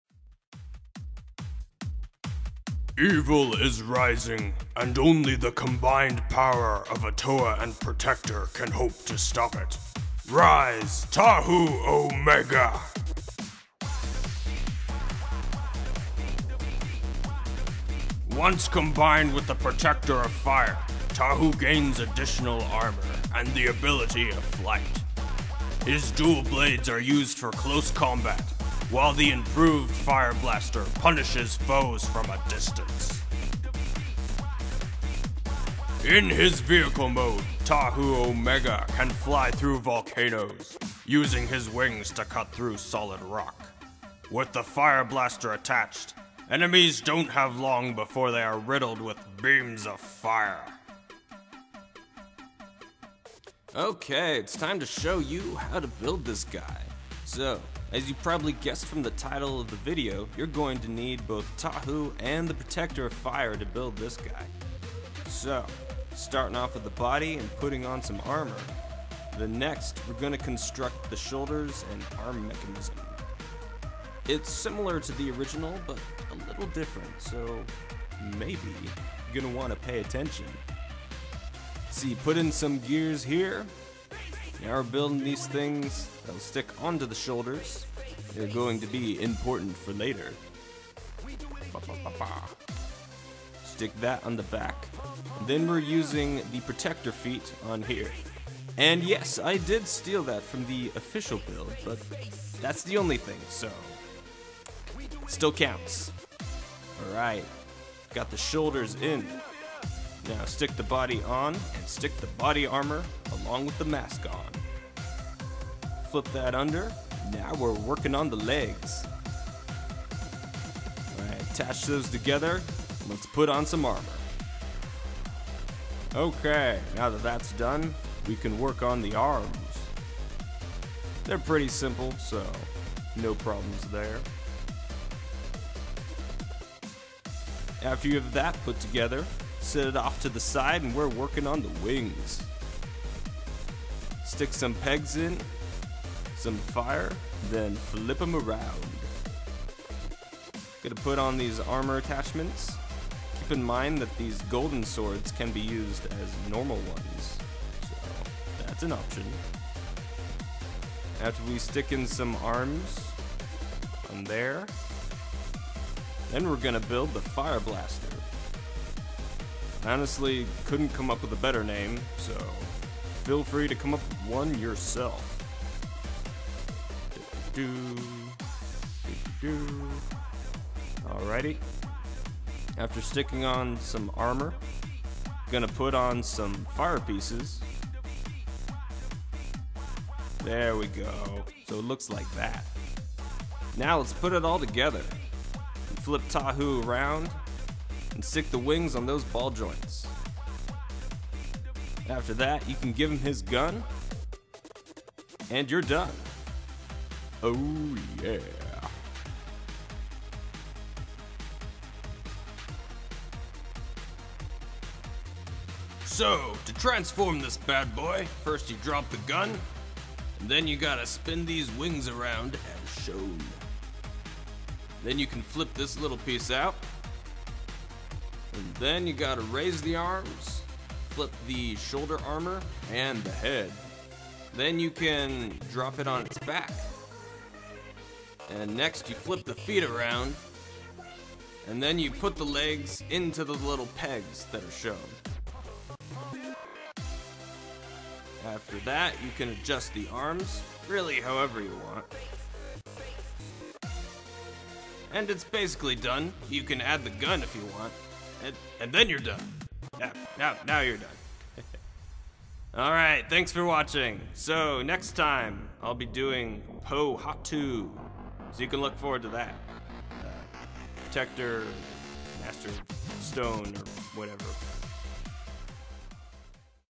Bioformer Reviews: Tahu Omega (Tahu / Protector of Fire)
He's finally here! Along with all the other Toa, soon...

Sets needed: Tahu Master of Fire and Protector of Fire

As you might have guessed, the name is based on "Omega Tahu", which was a nickname of 2015 Tahu. The other Toa will have their name with a Greek letter of my choosing next to it because I honestly couldn't come up with a better naming system.